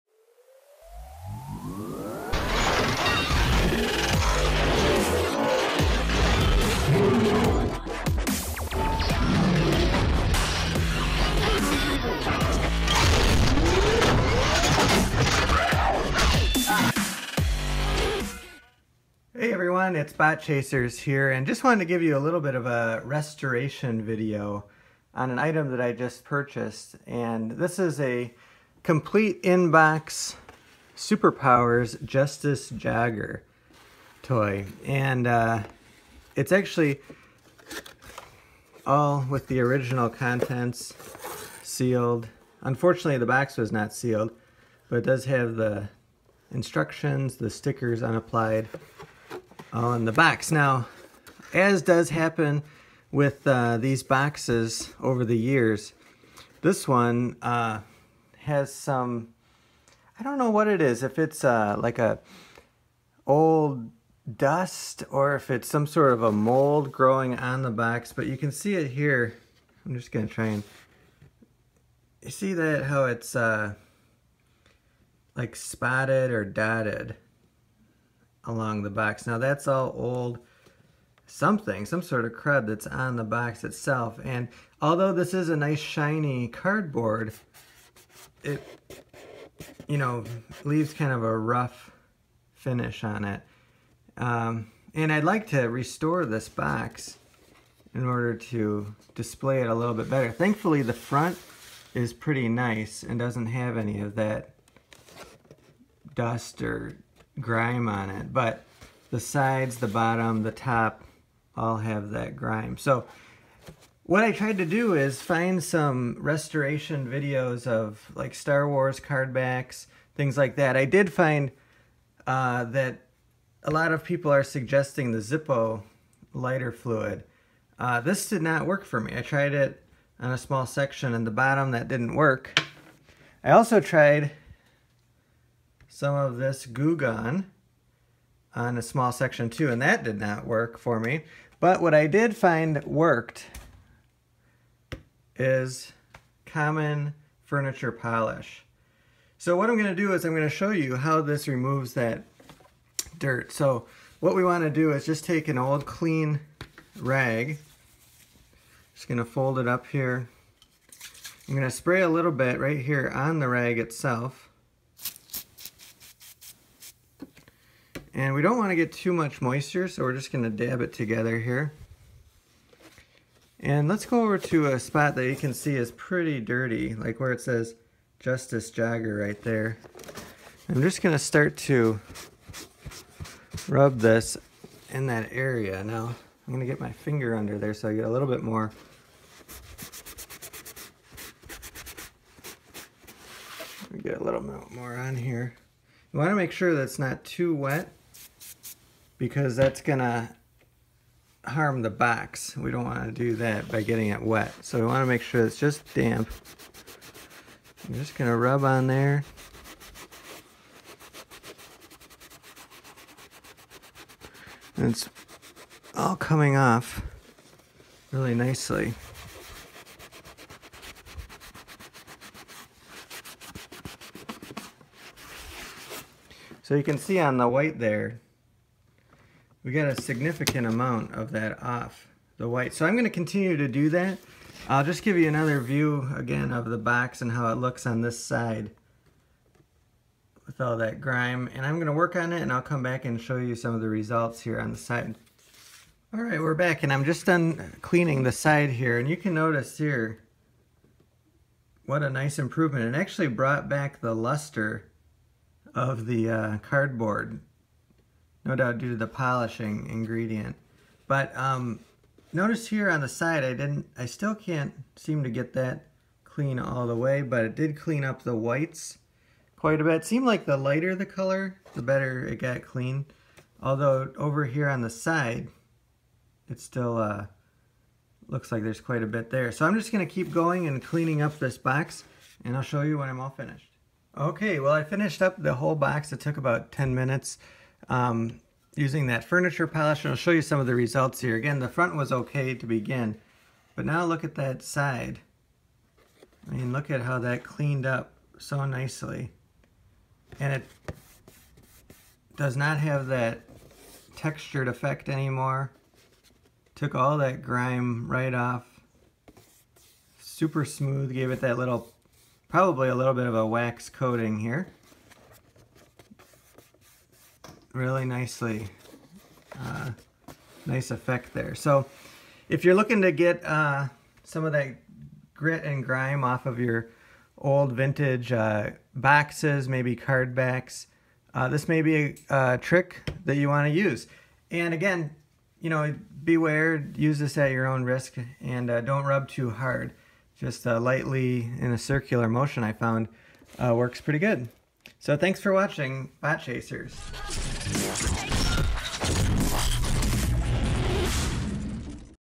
Clean Dirt and Mold off of Vintage Toy Boxes
In this video we take a look at how to clean that grit and grim off of your vintage toy boxes. The example we use is a Kenner Super Powers Justice Jogger box and some Pledge furniture wax.